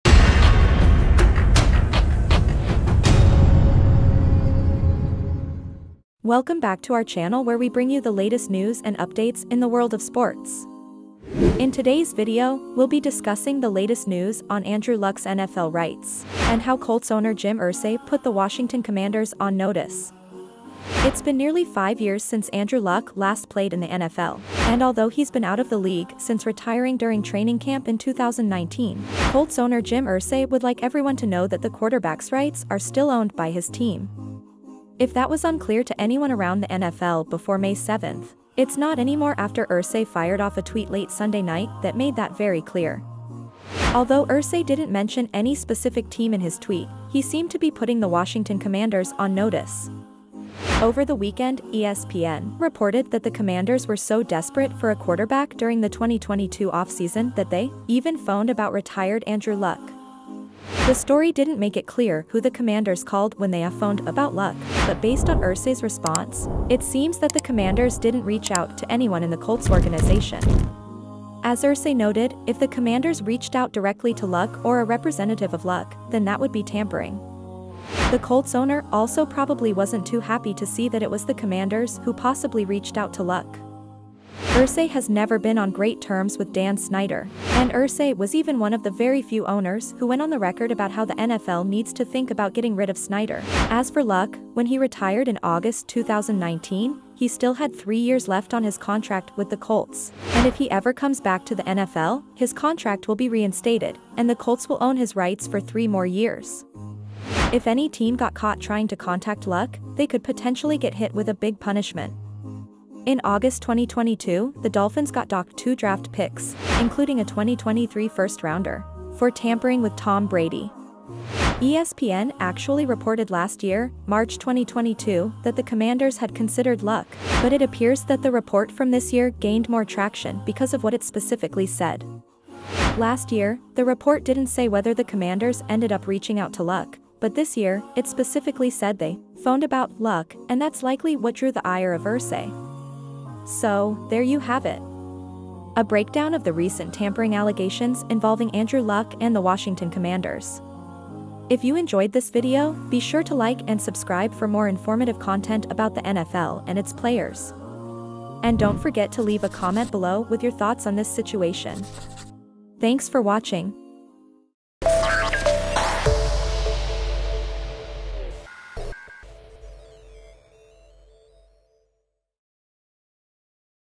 The Truth about Andrew Luck's NFL Rights What You Need to Know
In this video, we'll discuss the latest news on Andrew Luck's NFL rights and how Colts owner Jim Irsay fired off a tweet that put the Washington Commanders on notice. We'll also talk about the potential consequences of tampering and what it means for any team interested in Luck's return to the NFL.